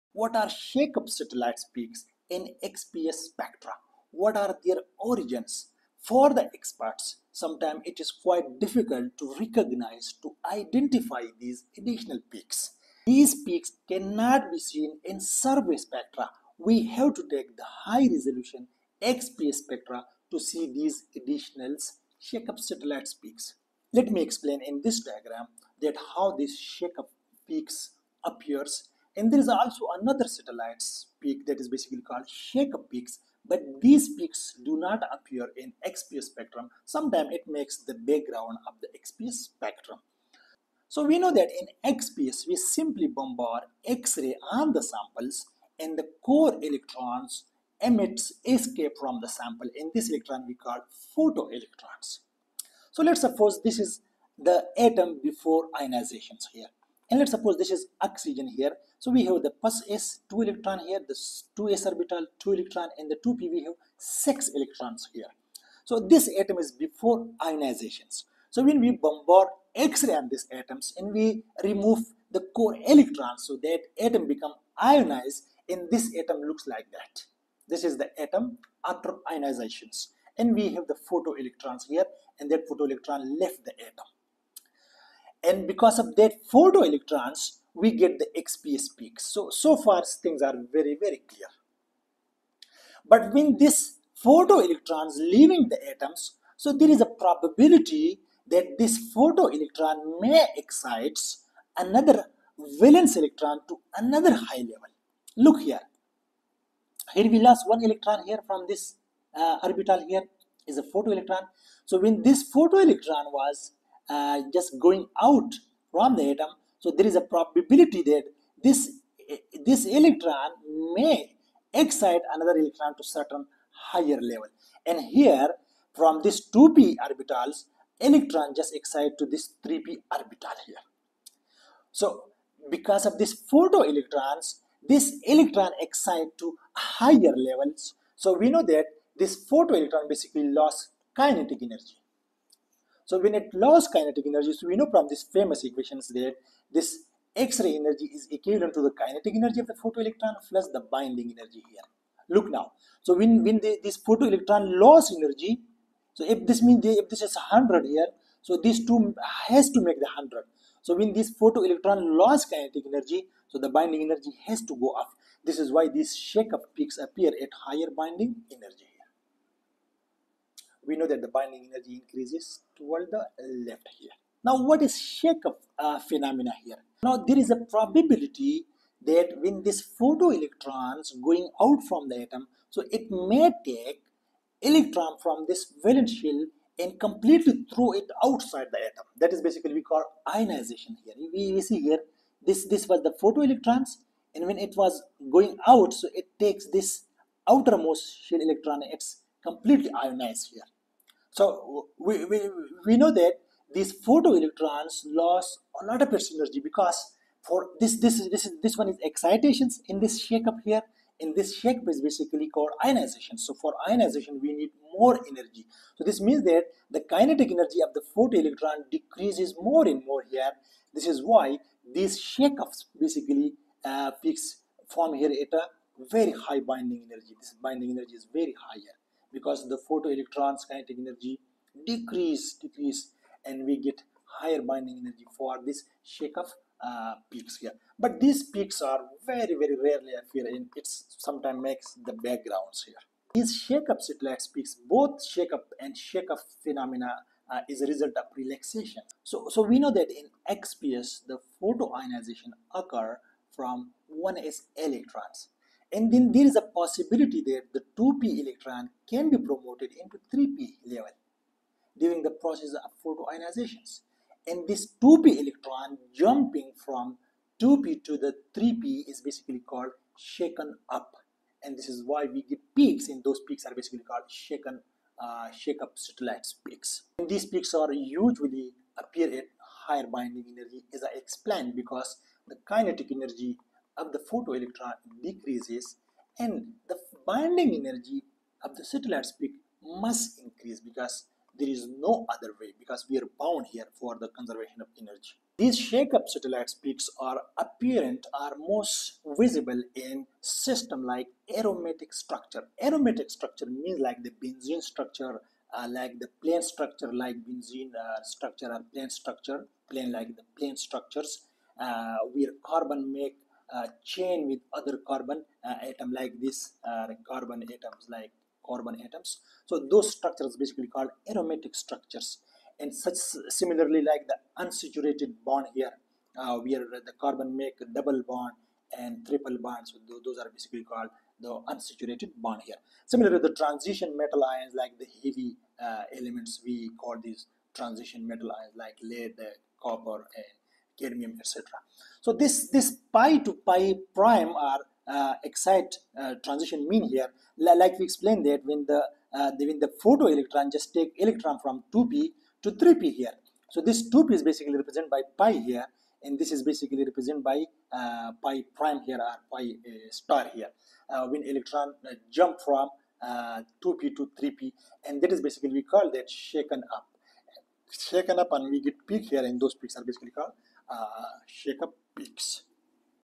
Shake-Up & Shake-Off Peaks Explained (XPS Satellites)
Shake-up and shake-off satellite peaks are often misunderstood in XPS analysis.
This video explains what they are, why they appear, and how they help reveal the electronic structure of elements.

In X-ray Photoelectron Spectroscopy (XPS), not all peaks come from direct photoemission.
Some arise from additional electron–electron interactions inside the atom.
These satellite peaks, known as shake-up and shake-off features — contain valuable information about oxidation states, valence electron configuration, and chemical bonding.

⭐ What Are Shake-Up Peaks?

Shake-up satellites occur when the photoelectron leaves the atom while simultaneously exciting another electron to a higher energy state.
This energy loss shifts the satellite peak to a higher binding energy than the main peak.

Shake-up peaks often appear for transition metals, oxides, and compounds with partially filled d- or f-orbitals.

⭐ What Are Shake-Off Peaks?

Shake-off satellites occur when the outgoing photoelectron transfers energy to another electron that is completely ejected from the atom.
This process produces broad, lower-intensity features further from the main peak.

Shake-off helps identify:

• strongly correlated electron systems
• multiplet splitting
• valence band interactions
• complex chemical states

⭐ Why These Peaks Matter

Shake-up and shake-off features provide insight into:

• oxidation states
• hybridization
• screening effects
• unpaired electrons
• electron–electron correlation

Understanding them is essential for interpreting XPS spectra in:

• metal oxides
• catalysts
• transition metal compounds
• nanomaterials
• surface chemistry